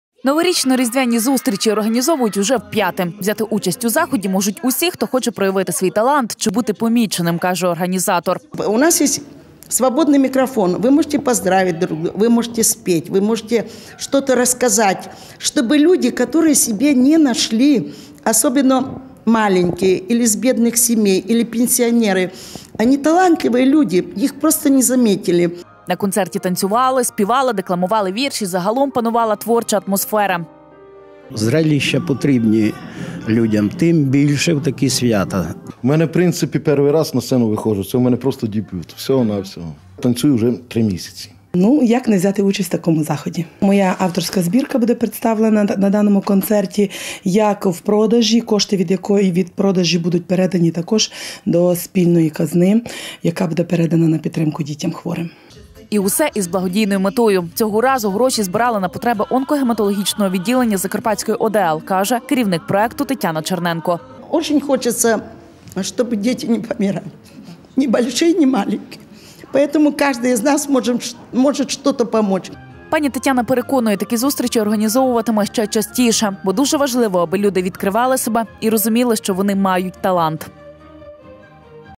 Новорічно-різдвяні зустрічі
У Мукачеві уже вп’яте відбулися новорічно-різдвяні зустрічі. Це благодійний проект, у якому може взяти участь кожен охочий: від малого до великого. На концерті танцювали, співали, декламували вірші. І усе – із благодійною метою: гроші збирали на потреби онко-гематологічного відділення Закарпатської ОДЛ.